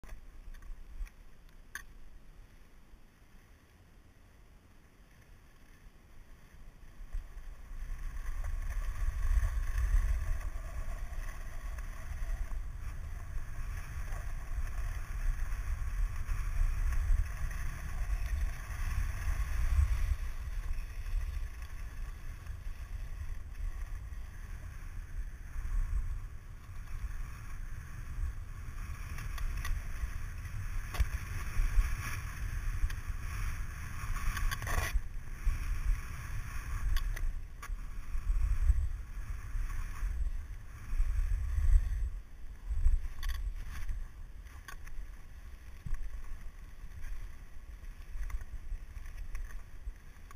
Skiing home from work
It was snowy and driving wasn't recommended. The bike didn't seem like a good way to get around either, so I turned to my trusty rock skis